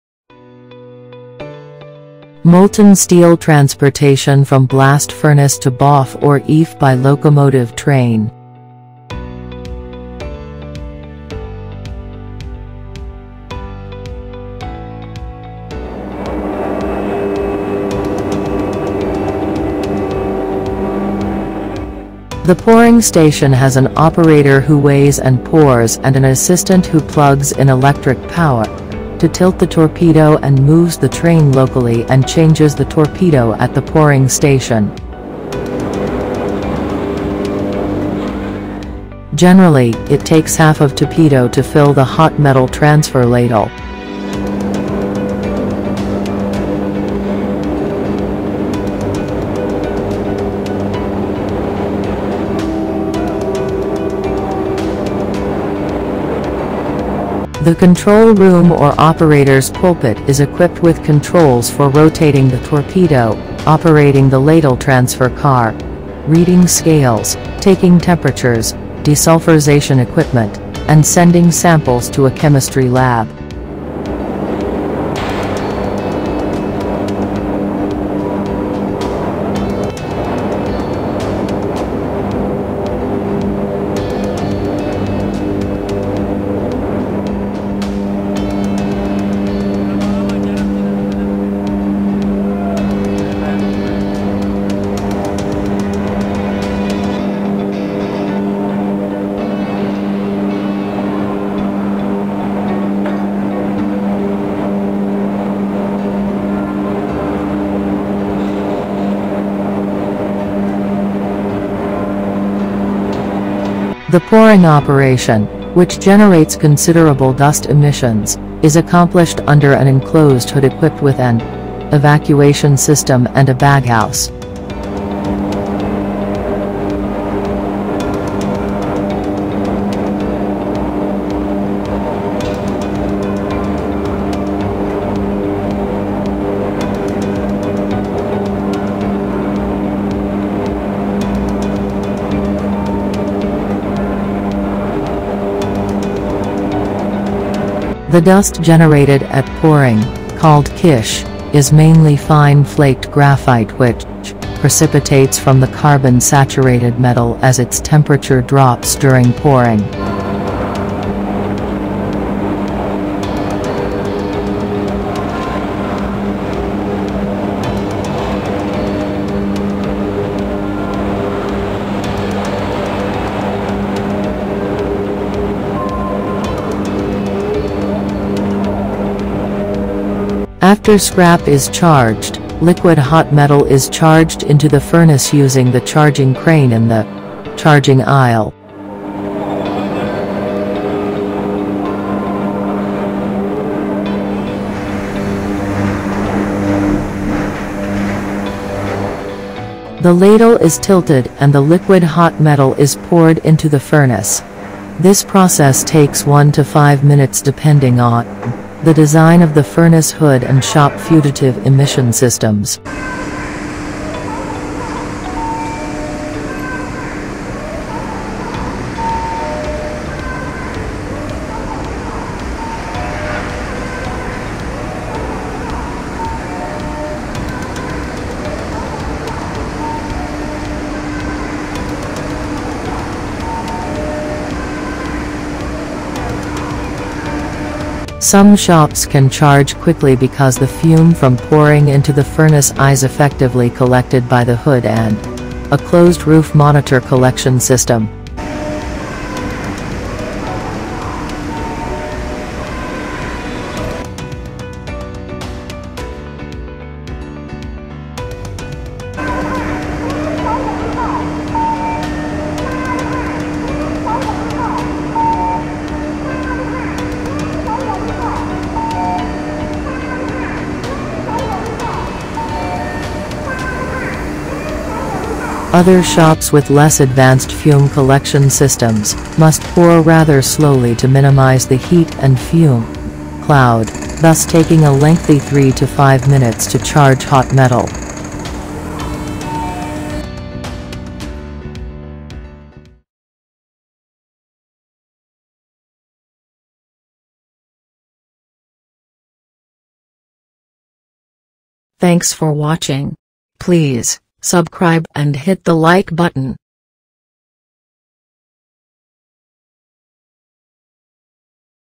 Hot Metal Ladle Transportation (Locomotive - Train)
Molten steel transportation from Blast Furnace to BOF or EAF by locomotive train.
The hot metal flows over the dam and down the iron runner where, by a series of gates, it is directed in sequence to the train of ladles positioned under stationary spouts along the runner. When casting is complete, the majority of blast furnaces keep the hot metal in the trough to enable more frequent casting, which is necessary to meet the rising production demands. This is now achievable because of advancements in installation equipment, methods, and through refractories. The iron troughs at a number of contemporary blast furnaces are detachable. Removable iron troughs reduce the amount of time needed to maintain the casting refractories, which increases the capacity of the furnace.

The Video content has been made available for informational & educational purposes only.

The Finest Hours Ch.